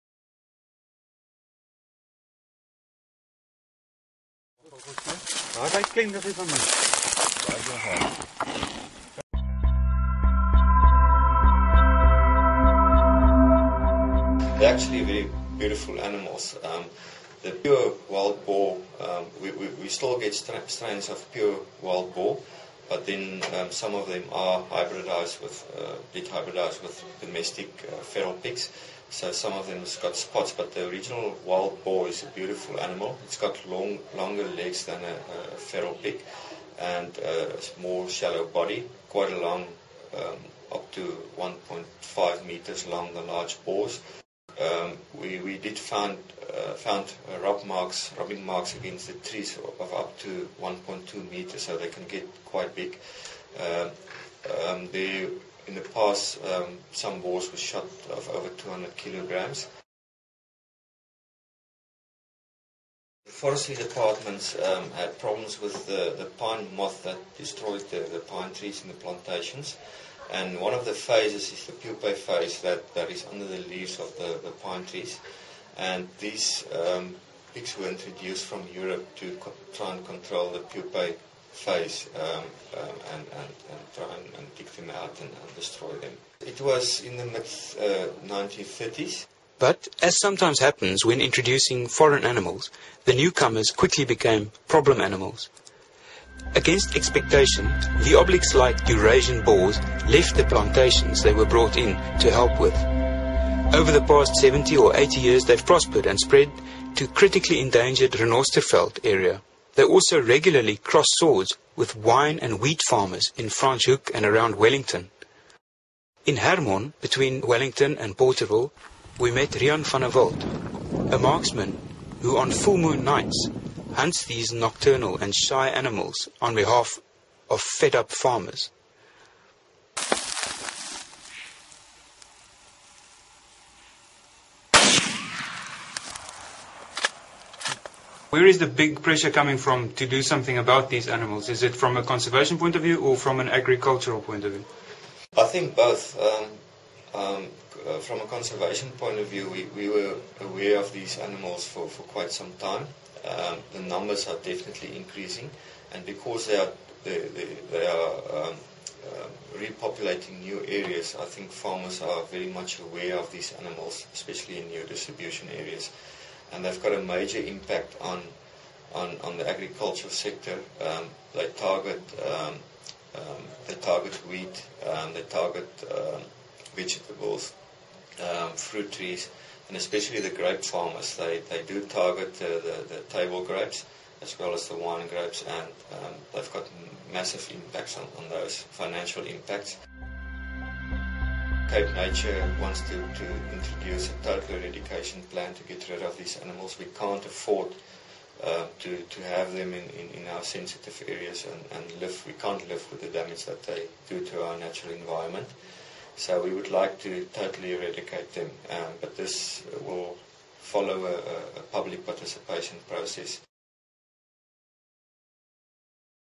Boar trouble....flv
They were brought in from Europe in the 1930s. But then things got out of hand.